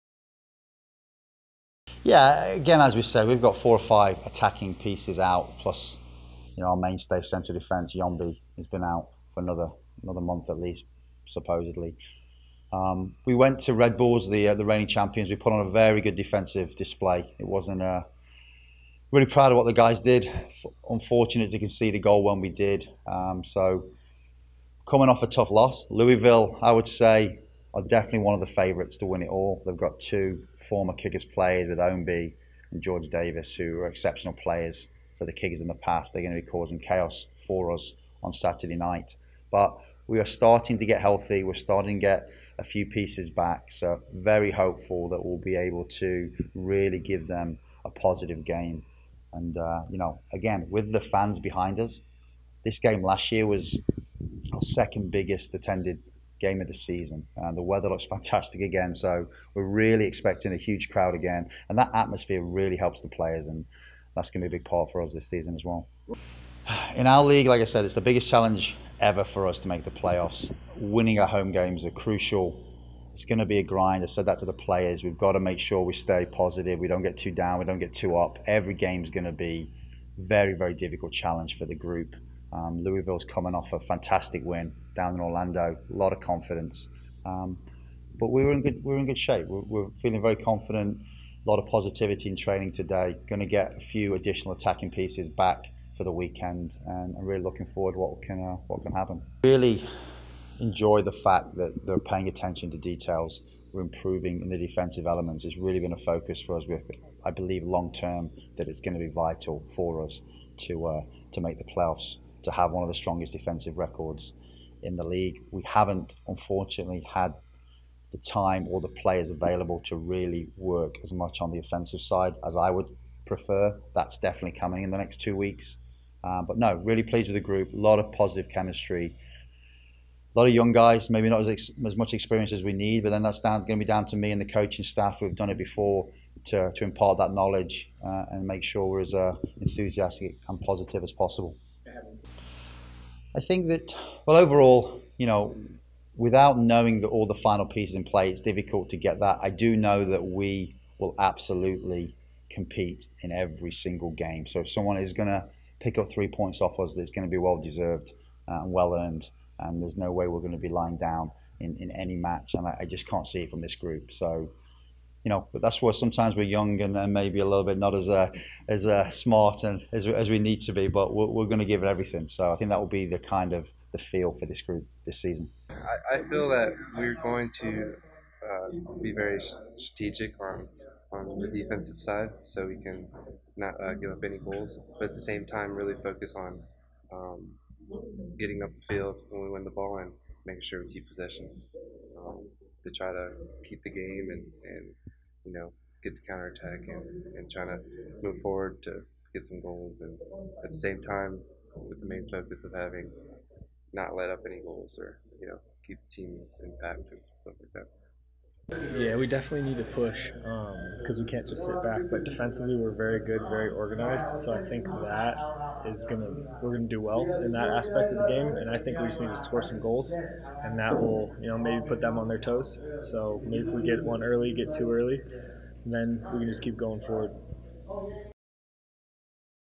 Louisville Preview Presented by Elephant Auto Insurance
Leigh Cowlishaw, Raul Gonzalez and Mikey Minutillo take a look at Saturday's match up against Louisville City FC.